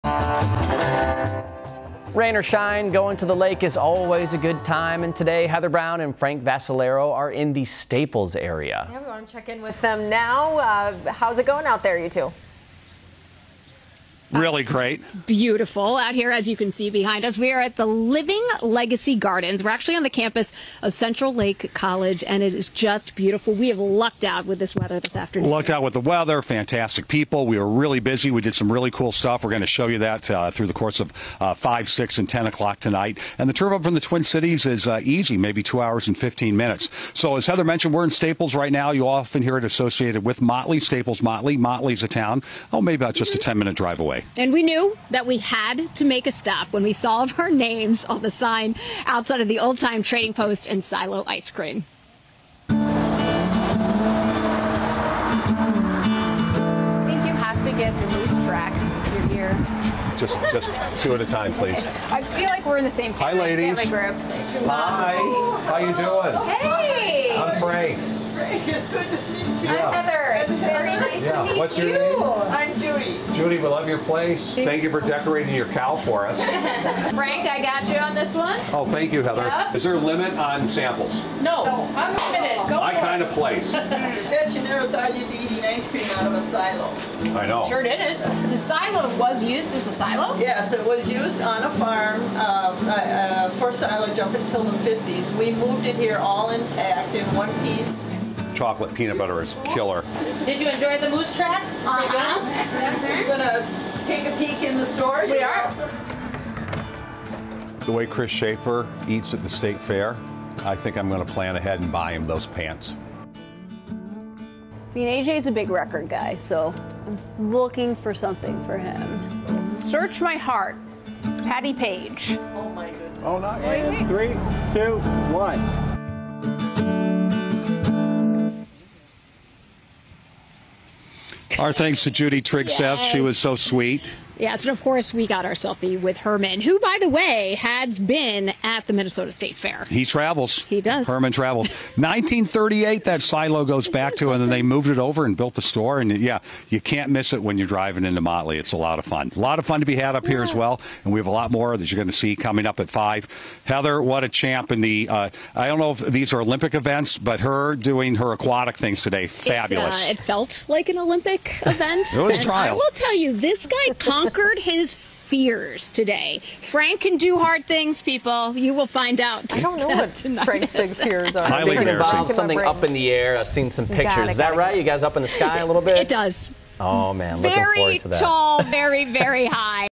A look at the Old Tyme Trading Post in Motley
Just 10 minutes from where Staples is it the town of Motley.